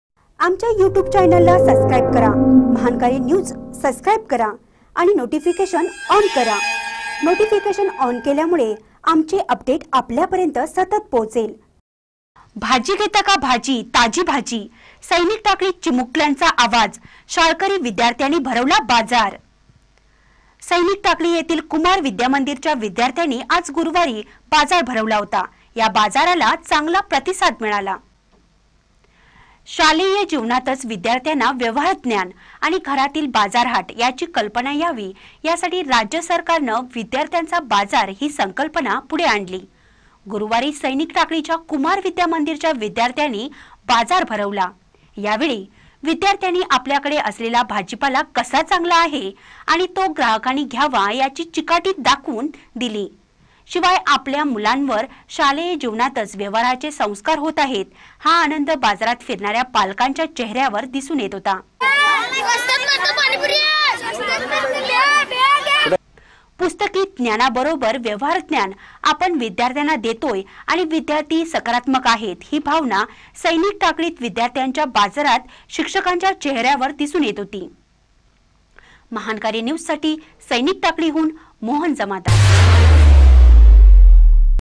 भाजी घेता का भाजी, ताजी भाजी, सैनिक टाकळीत चिमुकल्यांचा आवाज
भाजी घेता का भाजी, ताजी भाजी, सैनिक टाकळीत चिमुकल्यांचा आवाज, शाळाकरी विद्यार्थ्यांनी भरवला बाजार